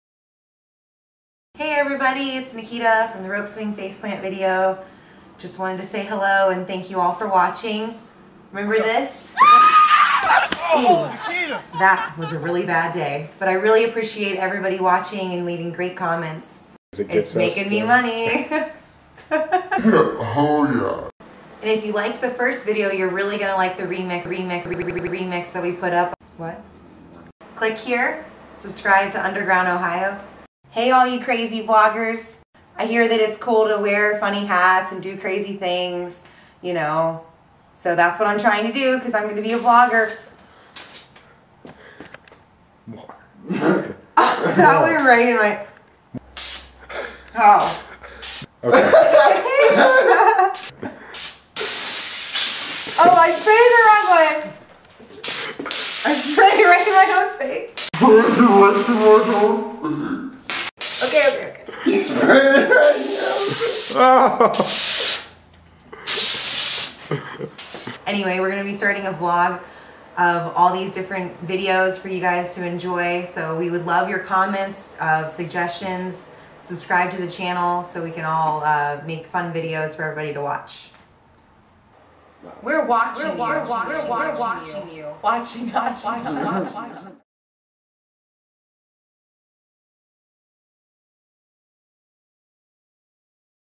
Rope swing faceplant- VLOG
Hey everyone! Nikita is back from the rope swing faceplant video and she is taking ideas for a VLOG series.